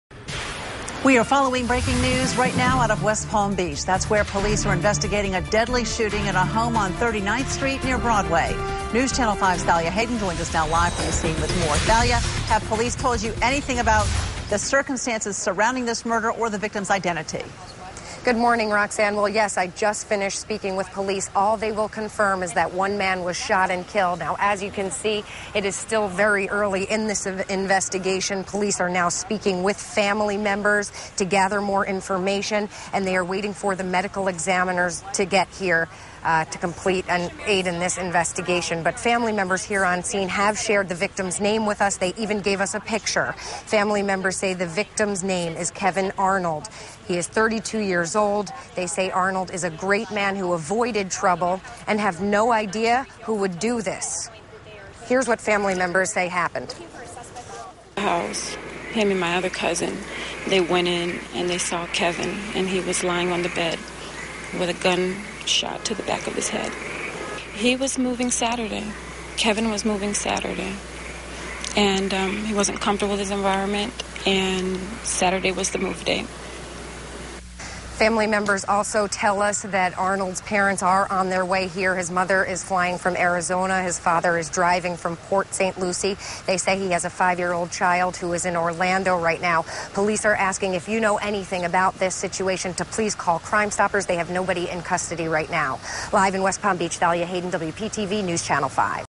West Palm Beach murder update
Man shot while sleeping in a home on 39th Street near Broadway